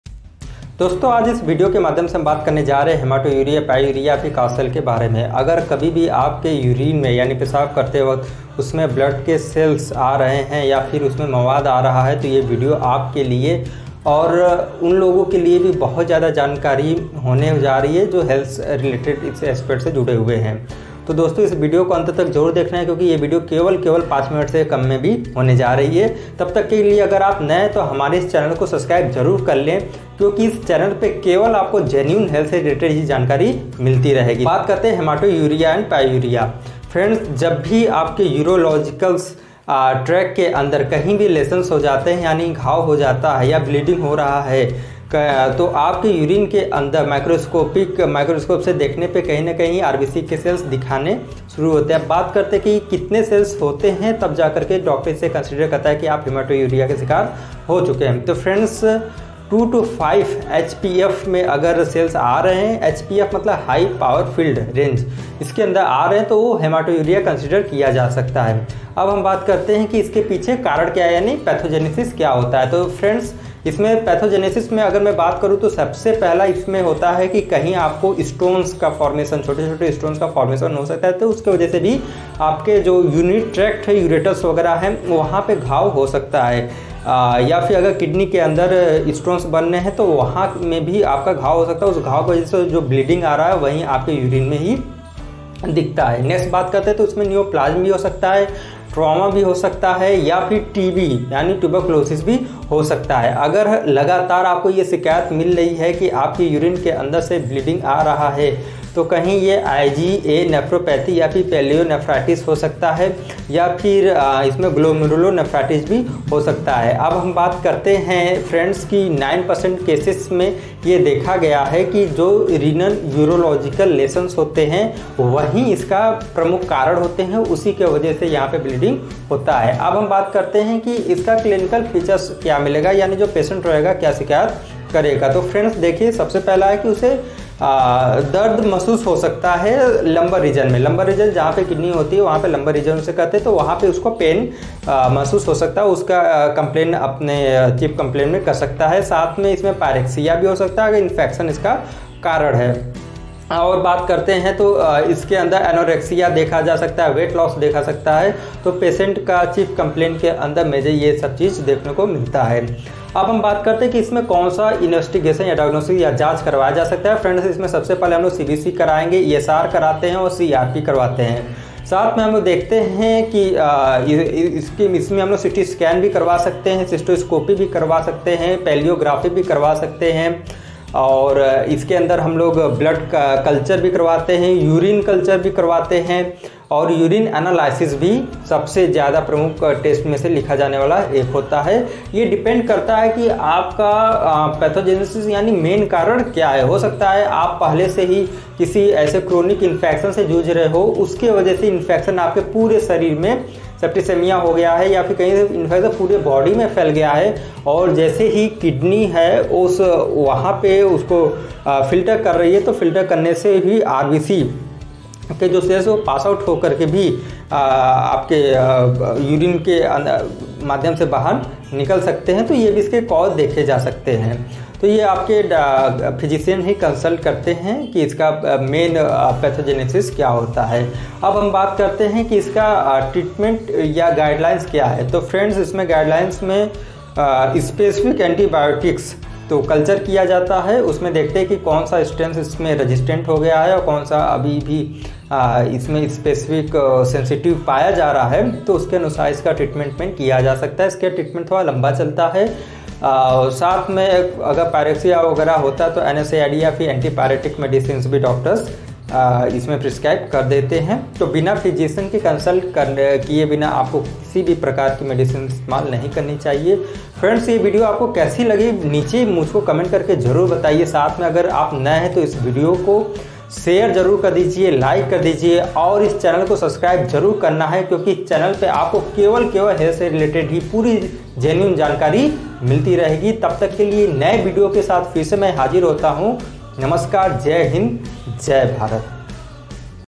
पेशाब में 🔥खून🔥 आने का कारण , इलाज । Hematuria cause and treatment.
In this video you will see detail about Hematuria it's cause , clinical sign and symptoms , investigation .
Hematuria , pyuria and casts cell can be present in urine . It indicates urinary tract infection. Hematuria is defined as two to five RBCs per high power field and can be detected by dipstick.
Hematuria commonly cause due to stone , neoplasms , tuberculosis , trauma . Hematuria can also occi due to acute cystitis , urethritis in women's can cause gross Hematuria .
Hematuria sign and symptoms can be lumbar pain , pyrexia, weight loss, anorexia etc.

Hematuria#Drshivamyadav#Hematuria#Drshivamyadav#